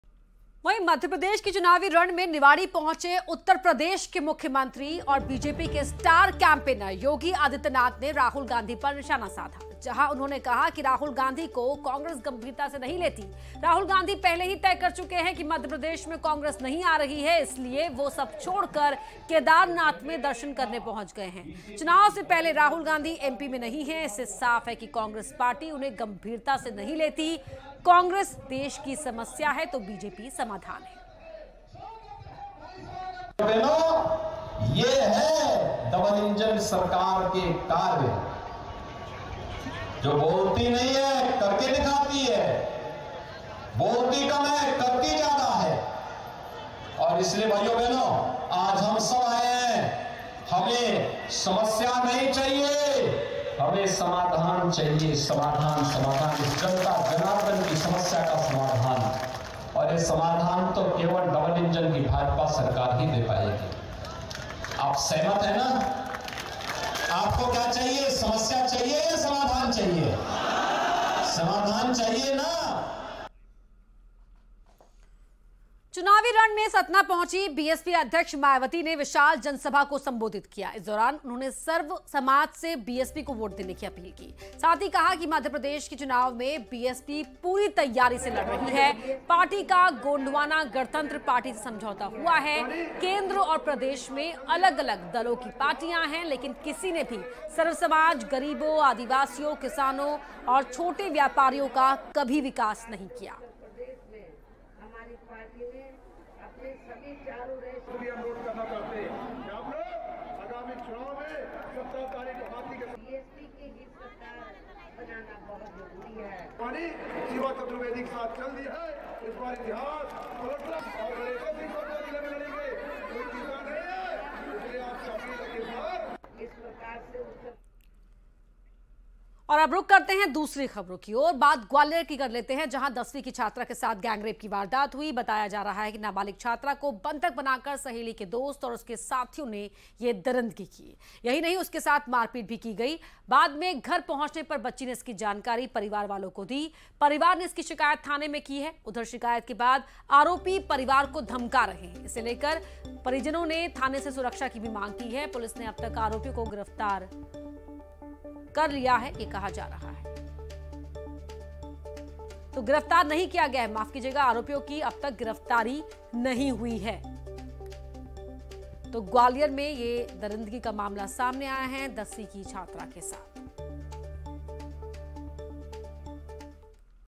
Yogi Adityanath Niwari Visit: MP के रण में सीएम योगी | MP Elections 2023 | CM Shivraj Singh
cmyogi yogiadityanath assemblyelection cgelection2023 bjpvscongress #